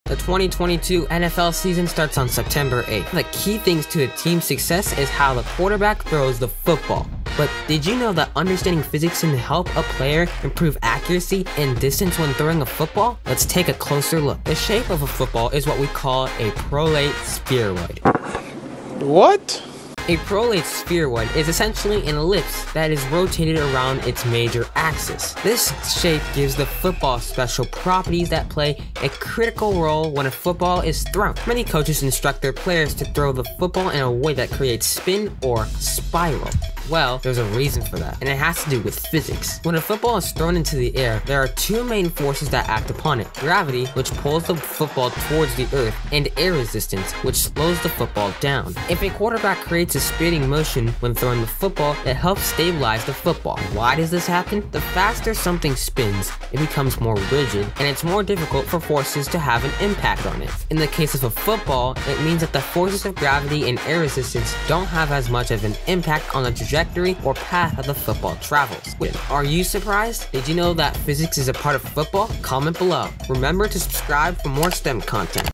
The Key to Winning Every Football Game
The 2022 NFL season starts on September 8th! One of the key thing to a team’s success is how the quarterbacks like Lamar Jackson and Aaron Rodgers throw the football! Did you know that understanding physics can help a player improve accuracy and distance when throwing a football?
Watch this engaging video to learn more!

Clips Used

Chapters

0:00 Intro
0:14 Shape of a Football
0:33 Why do Players use Spin in Their Football Throws?
0:54 Why is a Football More Stable When Subject to a Spin?
1:07 Putting it all Together
1:24 Outro